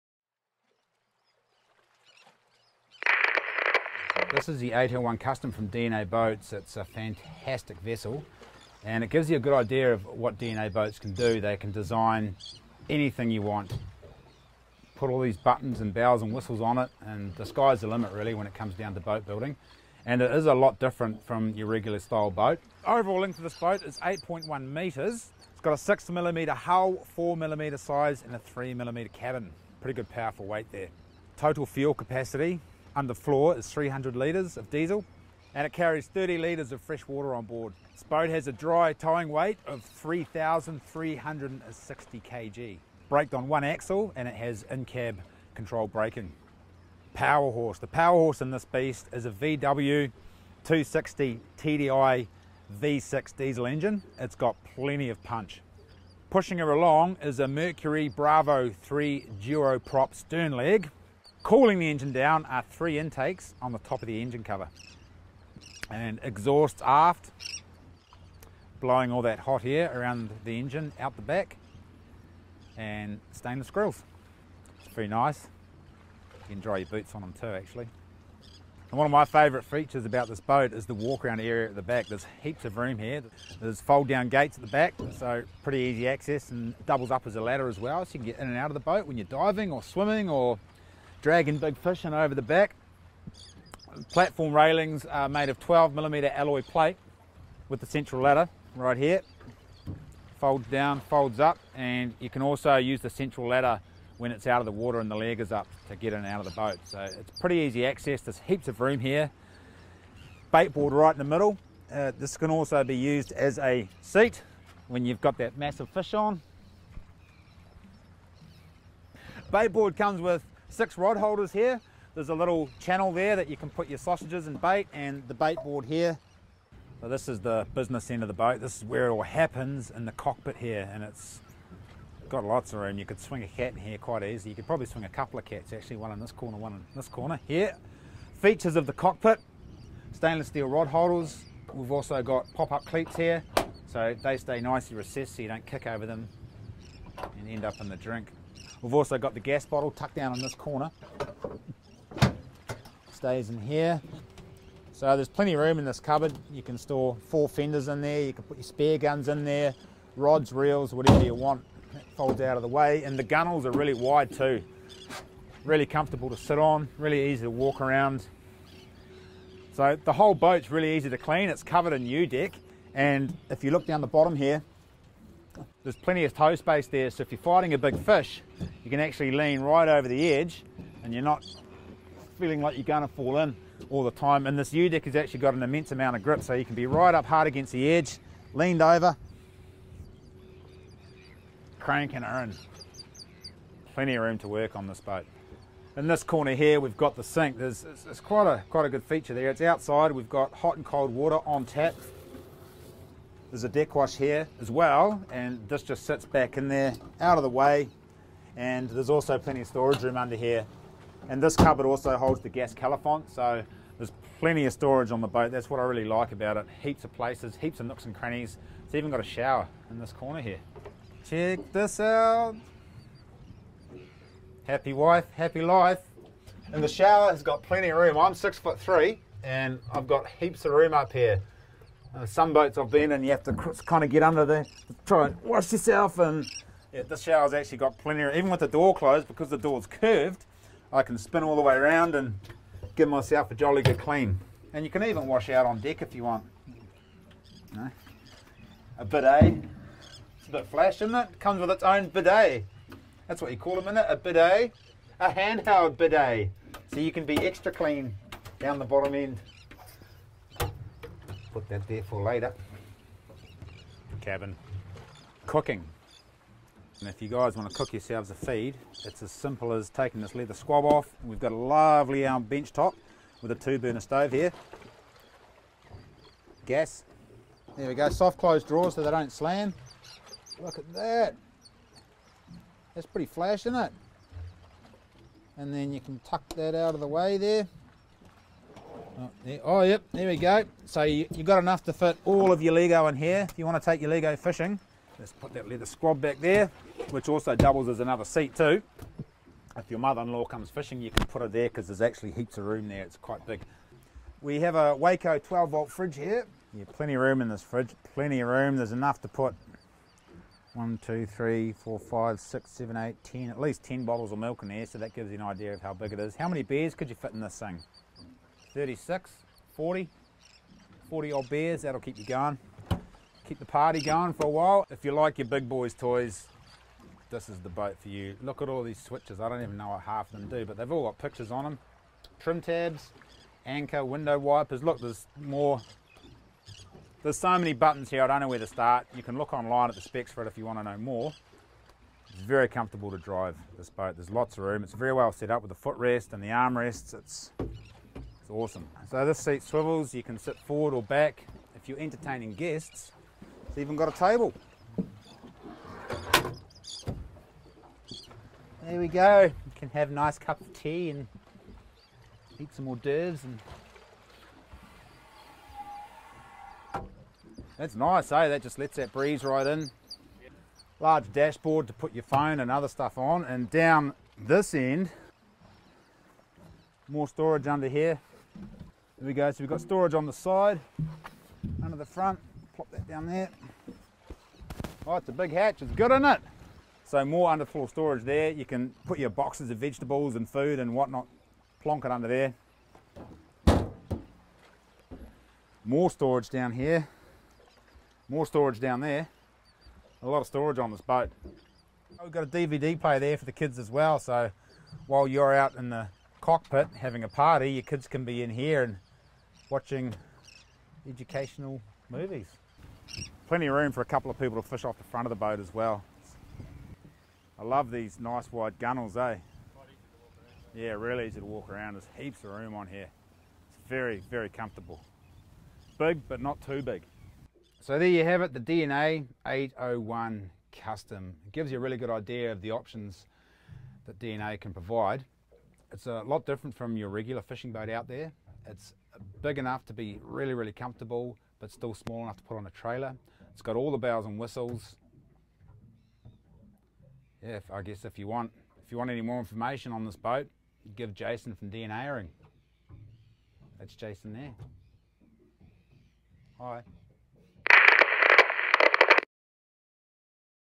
DNA BOATS 801 CUSTOM Introduction with Josh James
LENGTH 8.0 mtr overall
3.36 Ton towing weight (Dry)
6mm Hull
4mm Sides
3mm topsides
300 litre Diesel
30 litre freshwater
Height on trailer 3.5 mtr

THIS BUILD INCLUDES
Keel wear strip
Hella seahawk front and rear light bars
Webasto manual sunroof
Uniden TV
Bolster seats
Lenco Trim Tabs
Simrad 4G Radar
Simrad NSS16 Evo2
Airmar TM260 1 KW Transducer
Simrad AP24 Auto pilot
Mercury 260TDI Bravo 3 leg
Mercury Smart craft gauge
Lithium house battery system
Waeco Fridge
Bow thruster
Separate head/Shower
Gas califont
Cockpit sink and storage
Washdown pump
30L freshwater
300L diesel tank
Fully faired and epoxy paint.
S/S Bowrails
Fusion stereo and speakers- 2 x internal 2 x extremal
2 x wipers
Udek flooring
Cockpit shelves and 2 x fold down lockers
Rear transom platform with fold down ladder (central)
Cabin lined with carpet and genuine black leather
Front of cabin and lockers lined with noise 7mm insulating
foam under the carpet lining
Stress free drum anchor winch with freefall
Seadeck full length of gunnels
Hydraulic steering
Cabin underfloor storage
Cabin drawers (painted)
DNA tandem alloy trailer with Hydrastar cab controlled
electric/hydraulic braking
Front Vee berth squabs
Vee berth under squab storage

Bifold cabin door
Sliding side windows
Gas cook top and bench
Fold out table.